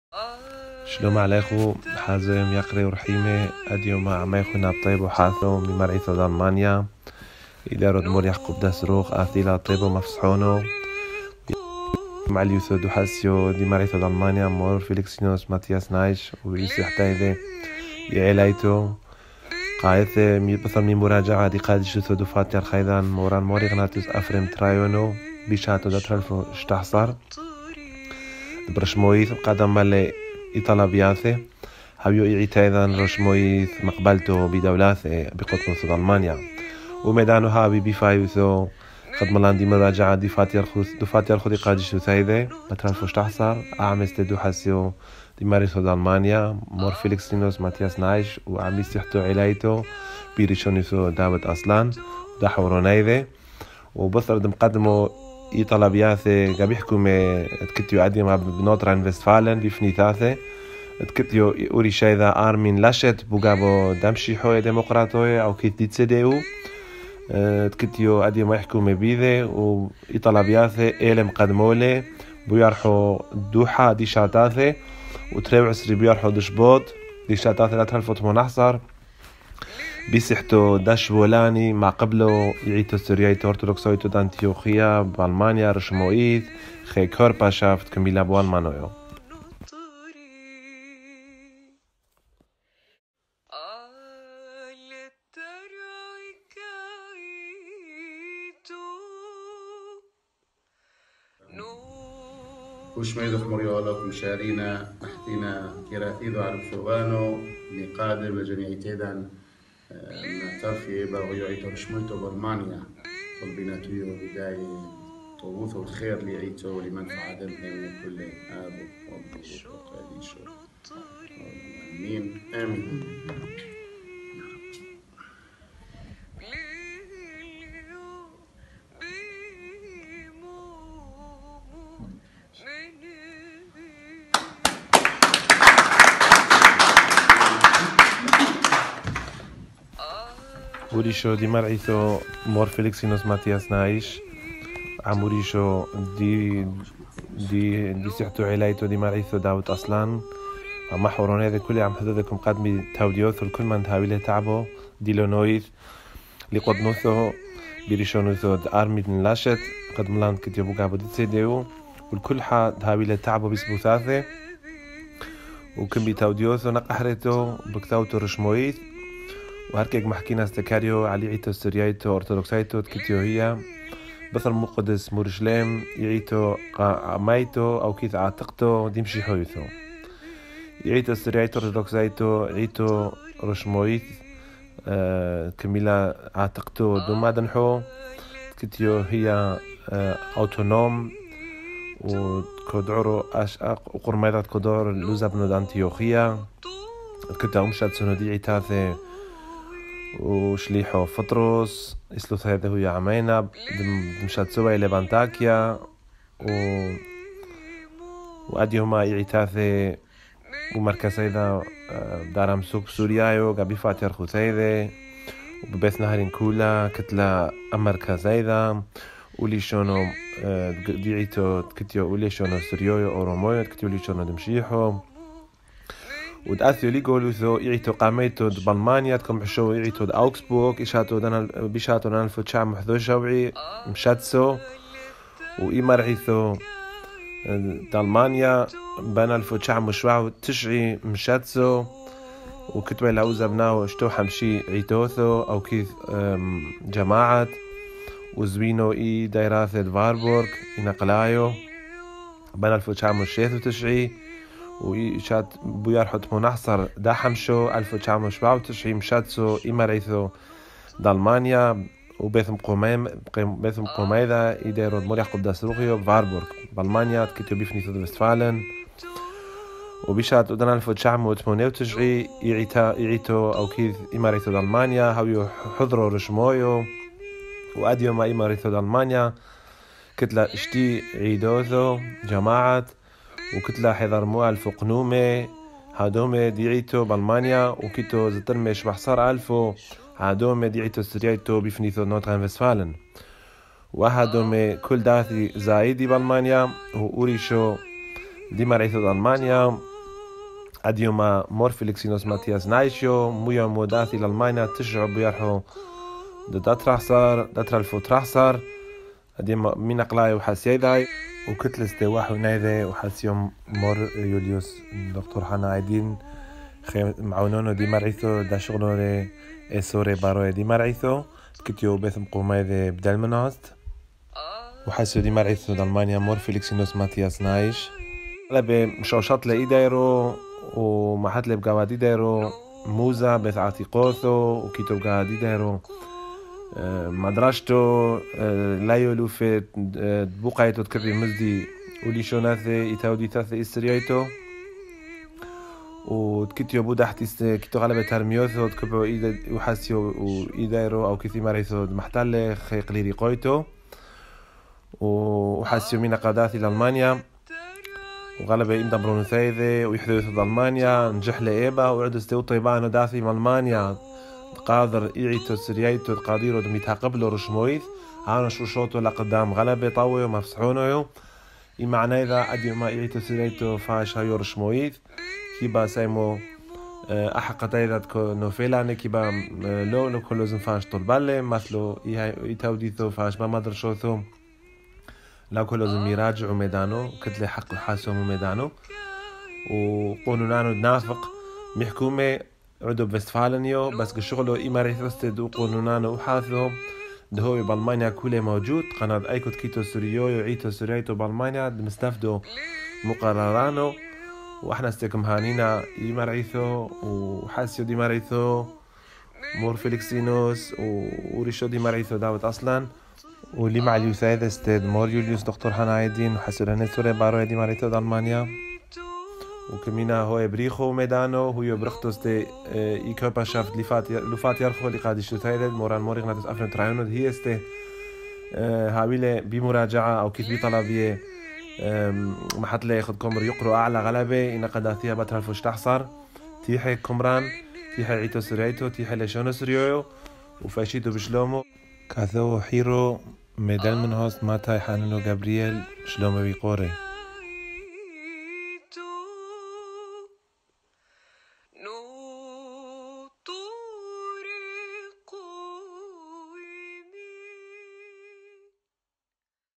Syrisch-orthodoxe Kirche wird Körperschaft 2/2018
in NRW wird die Syrisch-orthodooxe Kirche Körperschaft unter der Leitung von Erzbischof Mattias Nayis und Diözesanvorsitzender David Aslan